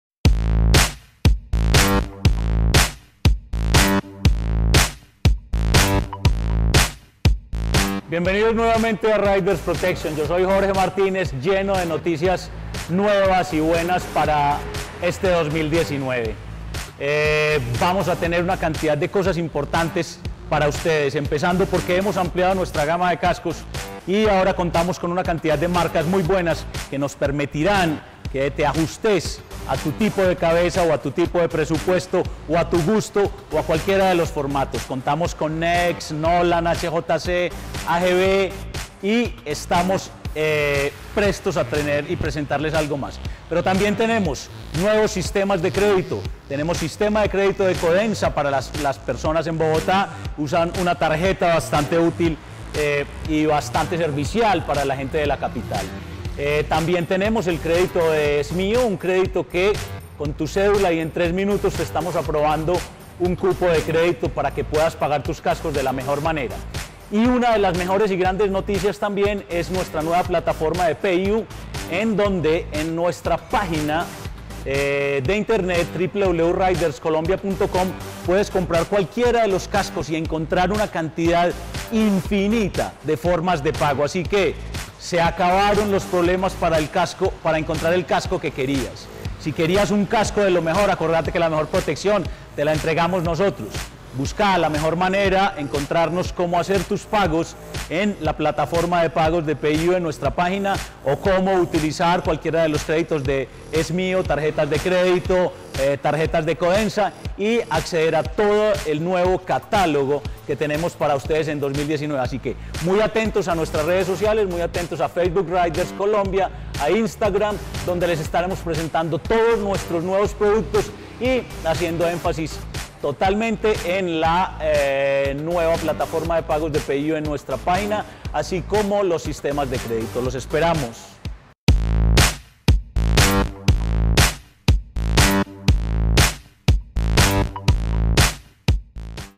Riders gran variedad de gama de cascos
Contamos con una gran variedad de marcas como NOLAN, AGV, HJC entre otras, las cuales permiten que te acomodes según tu presupuesto, gusto y comodidad.

Y tú... ¿Ya eres Riders?
WPP a nivel nacional: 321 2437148
Medellín: 5017348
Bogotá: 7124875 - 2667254